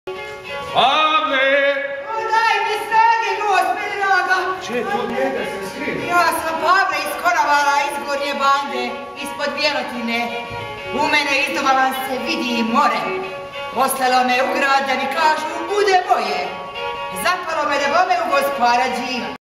Pavle i Đivo - edukativne maskote Dubrovačkih muzeja
Generalna proba u žitnici "Rupe" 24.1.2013. u 13 sati - za Noć muzeja. Glorija Šoletić kao Pavle i Edi Jertec kao Đivo.